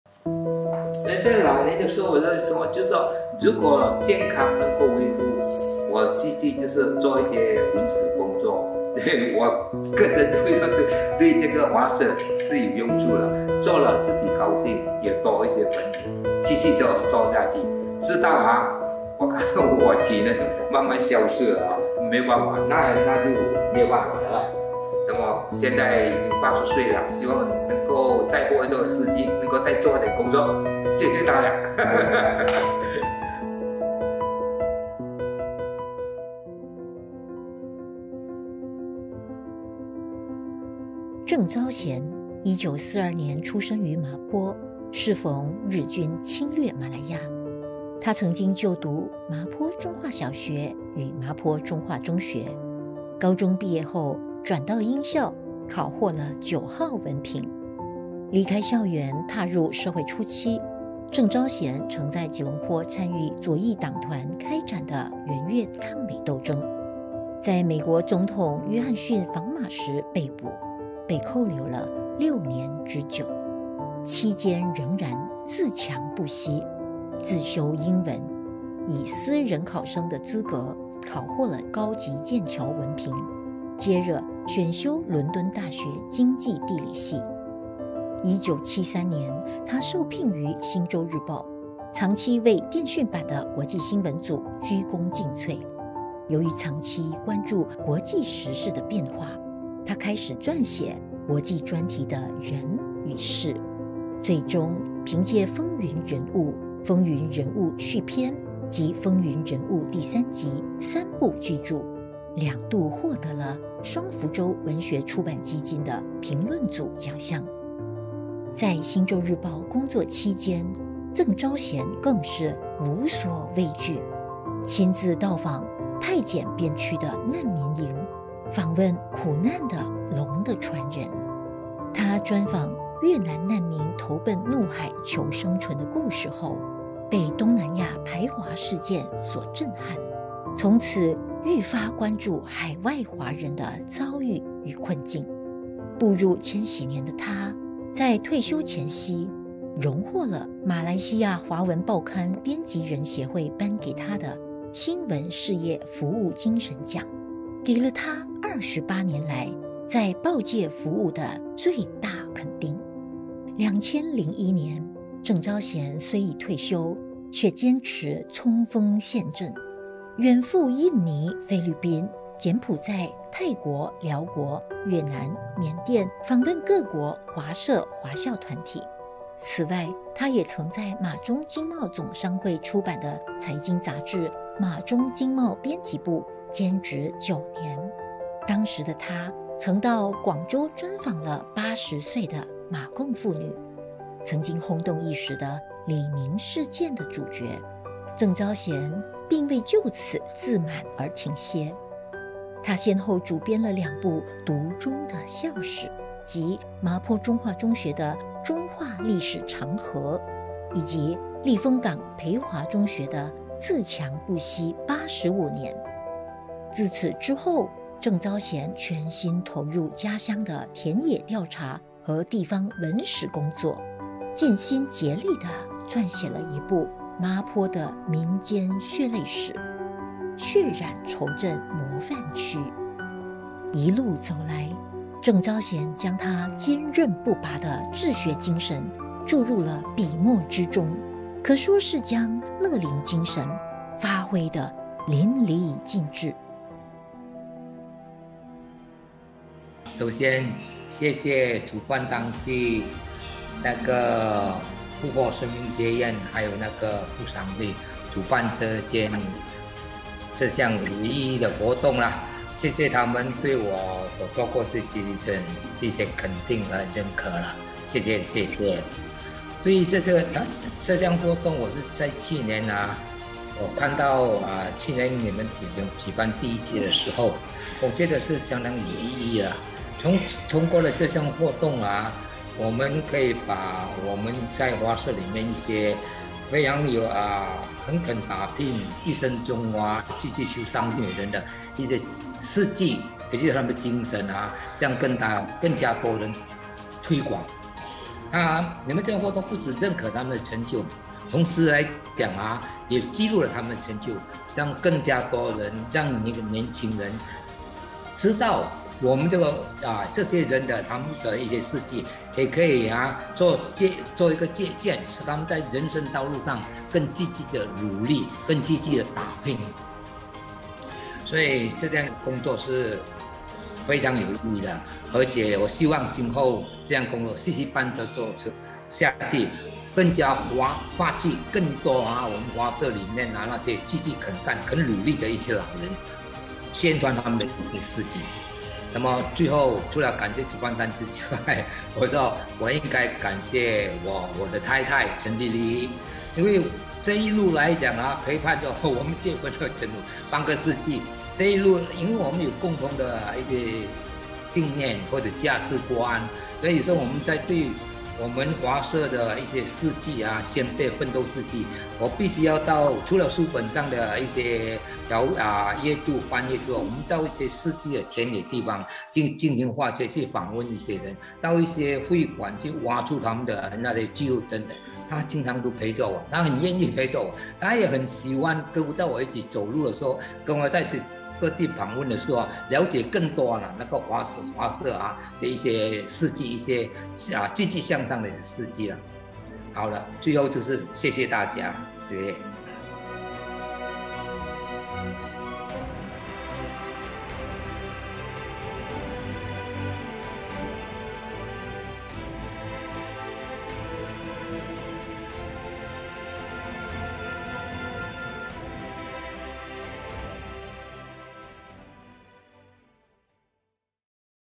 《告别扣留营走入报界》—— 郑昭贤 | 不惑乐龄精神奖得主
资深媒体人郑昭贤于1942年在麻坡出世。出世时，适逢日军侵略马来亚，今年郑昭贤80岁，也是日军攻占麻坡80周年。他在《不惑》创刊号写了《田野调查，探索麻坡二战史》。

作为报人，他十分关注东南亚各地华人的命运与遭遇，写了不少有关的报导。晚年，他专注于家乡麻坡的地方史，搜寻麻坡先辈的奋斗事迹，出版了几本麻坡史书。

他离开服务28年的星洲日报之后的新路程，于12年前开始开田野调查工作，撰写地方史、及人物与事件，颇有收获，而且还会继续下去。

完整文章介绍请到（2021不惑老人日暨乐龄精神奖特辑）：

* 若您想报名我们的『老爸老妈来上课』免费公益网课，欢迎联系/Whatsapp我们：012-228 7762